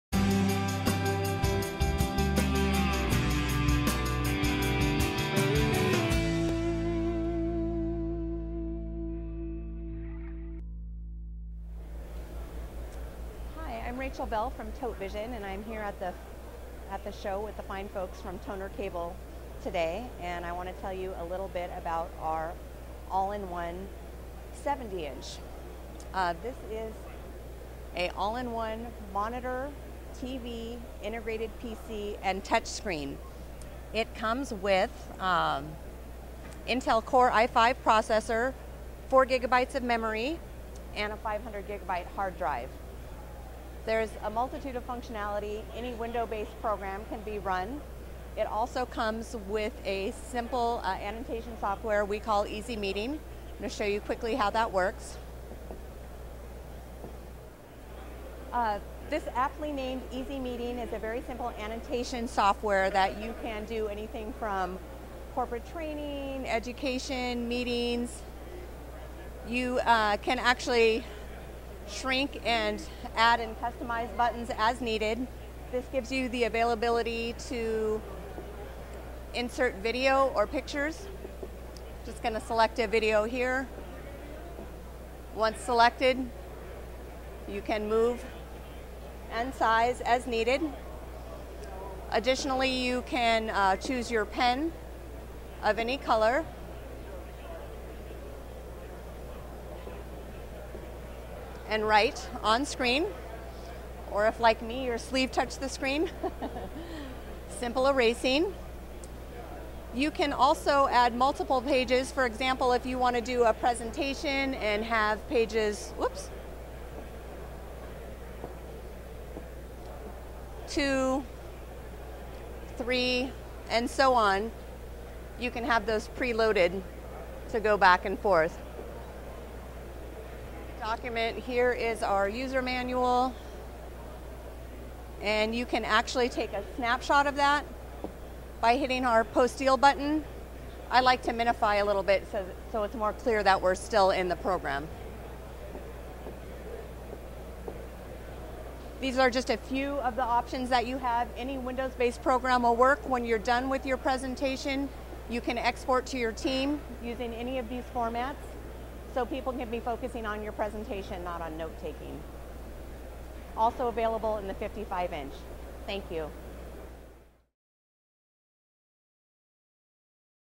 Totevision 70" Touch Screen LED Monitor / TV / PC - Toner Cable
Rachel Bell from Totevision show us some great features on the 70" touchscreen Monitor, TV, and PC all in one. Available at Toner Cable.

Transcript:

Hi, I'm Rachel Bell from Totevision and I am here at the show with the fine folks from Toner Cable today and I want to tell you a little bit about our all in one, 70 inch. This is an all in one monitor/TV/Integrated PC and touch screen.

It comes with Intel Core i5 processor, 4GB of memory, and a 500 GB hard drive. There is a multitude of functionality, any window based program can be run. It also comes with a simple annotation software we call easy meeting. I am going to quickly show you how that works.

This aptly named easy meeting is a very simple annotation software that you can do anything from corporate training, education, meetings. You can actually shrink, add and customize buttons as needed. This gives you the availability to insert video or pictures. Just going to select a video here. Once selected, you can move and size as needed.

Additionally you can choose your pen of any color and write on screen or, if like me and your sleeve touched the screen, simple erasing. You can also add multiple pages. For example, if you want to do a presentation and have pages 2, 3, and so on. You can have those preloaded and go back and forth.

Here is our user manual. And you can actually take a a snapshot of that by hitting our postil button. I like to minimify a little bit so it's more clear that we are still in the program. These are just a few of the options that you have, any windows based program will work. When you are done with your presentation, you can export to your team using any of these formats. So people can be focused on your presentation and not note taking.

Also available in the 55 inch. Thank you.